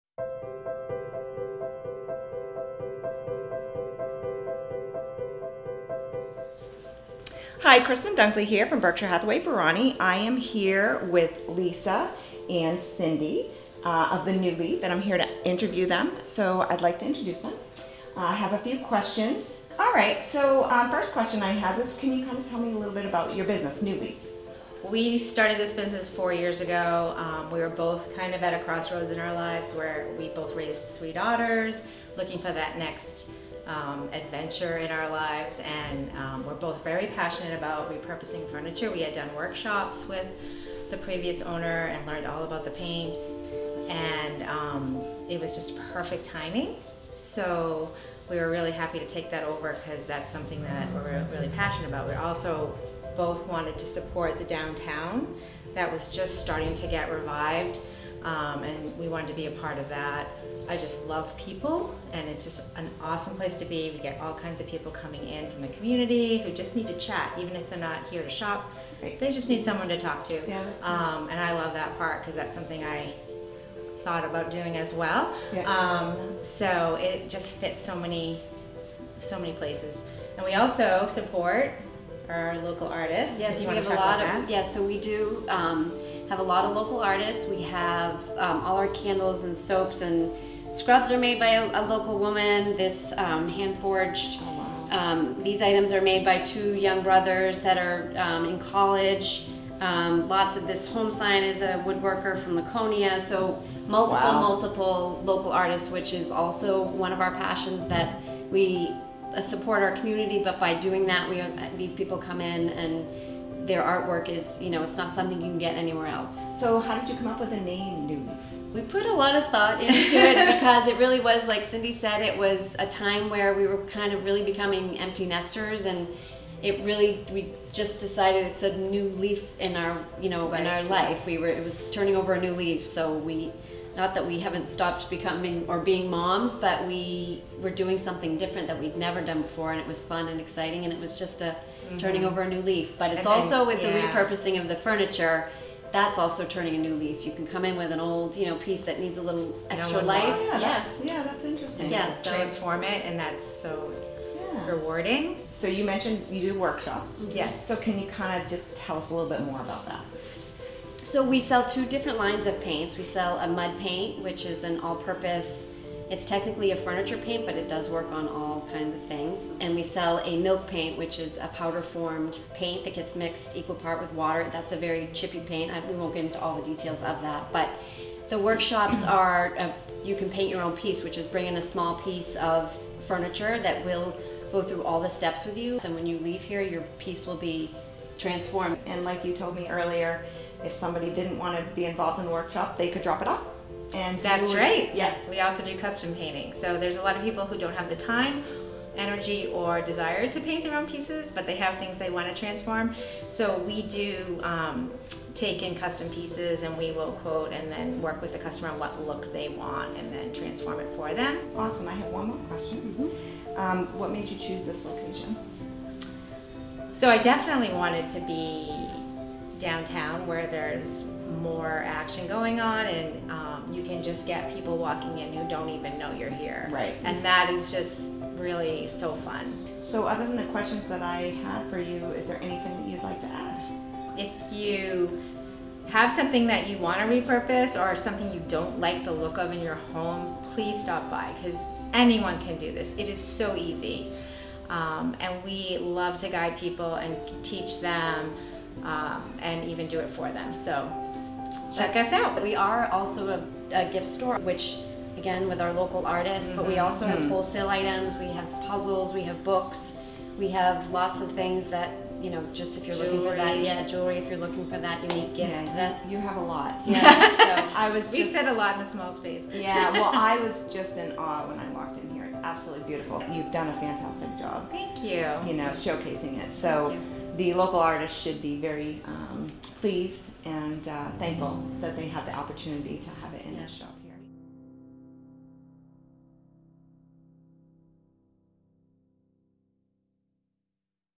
Meet Cindy Tomlinson and Lisa Harris owners of New Leaf at 600 Main St. in downtown Laconia, NH
Meet Cindy Tomlinson and Lisa Harris owners of New Leaf located at 600 Main St. in downtown Laconia, NH